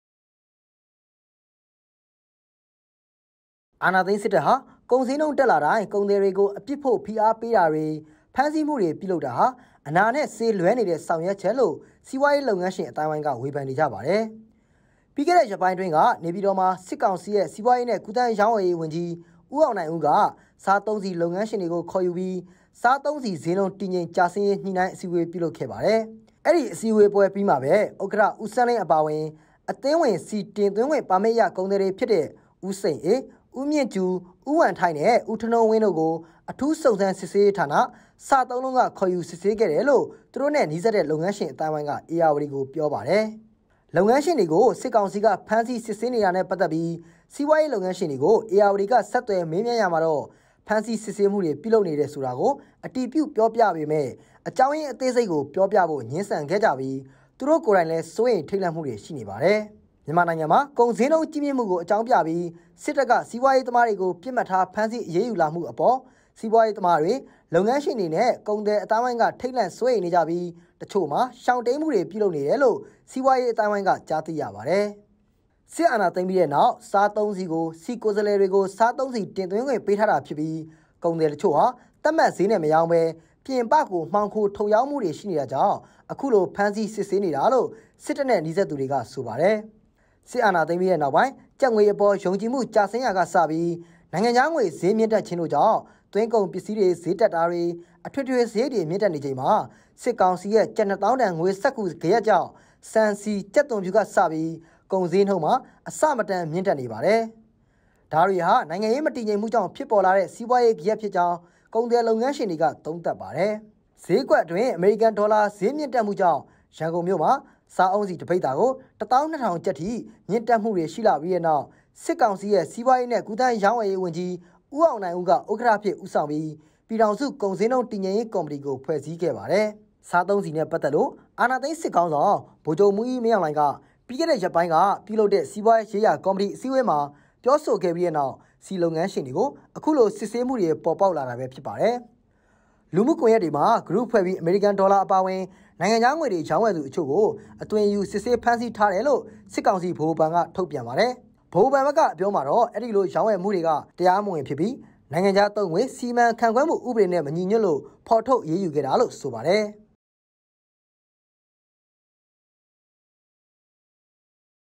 ကုန်သည်တွေကိုဖမ်းတော့ ကုန်စျေးနှုန်းက ကျဆင်းသွားမှာလား
ကုန်သည်တွေကိုဖမ်းတော့ ကုန်စျေးနှုန်းက ကျဆင်းသွားမှာလား(ရုပ်/သံ)

ဧရာ၀တီ၊ စက်တင်ဘာ ၄၊ ၂၀၂၃

မြန်မာနိုင်ငံမှာ ကုန်စျေးနှုန်းကြီးမြင့်မှုကို အကြောင်းပြုပြီး စစ်တပ်က စီးပွားရေးသမားတွေကို ပစ်မှတ်ထား
ဖမ်းဆီးအရေးယူလာမှုအပေါ် စီးပွားရေးသမားတွေ၊ လုပ်ငန်းရှင်တွေနဲ့ ကုန်သည်အသိုင်းအဝိုင်းက ထိတ်လန့်စိုးရိမ်နေကြပြီး တချို့မှာ ရှောင်တိမ်းမှုတွေ ပြုလုပ်နေတယ်လို့ စီးပွားရေးအသိုင်းအဝိုင်းက ကြားသိရပါတယ်။

အာဏာသိမ်းစစ်တပ်ဟာ ကုန်စျေးနှုန်းတက်လာတိုင်း ကုန်သည်တွေကို အပြစ်ဖို့ဖိအားပေးတာတွေ၊ ဖမ်းဆီးမှုတွေလုပ်တာဟာ အနာနဲ့ဆေး လွဲနေတဲ့ ဆောင်ရွက်ချက်လို့ စီးပွားရေးလုပ်ငန်းရှင်အသိုင်းအဝိုင်းက ဝေဖန်နေကြပါတယ်။

လုပ်ငန်းရှင်တွေကို စစ်ကောင်စီက ဖမ်းဆီးစစ်ဆေးနေတာနဲ့ ပတ်သက်ပြီး စီးပွားရေးလုပ်ငန်းရှင်တွေကို ဧရာ၀တီက ဆက်သွယ်မေးမြန်းရာမှာတော့ ဖမ်းဆီးစစ်ဆေးမှုတွေ ပြုလုပ်နေတယ်ဆိုတာကို အတည်ပြုပြောဆိုပေမယ့် အကြောင်းအရင်း အသေးစိတ်ကို ပြောပြဖို့ ငြင်းဆန်ခဲ့ကြပြီး သူတို့ကိုယ်တိုင်လည်း စိုးရိမ်ထိတ်လန့်မှုတွေ ရှိနေပါတယ်။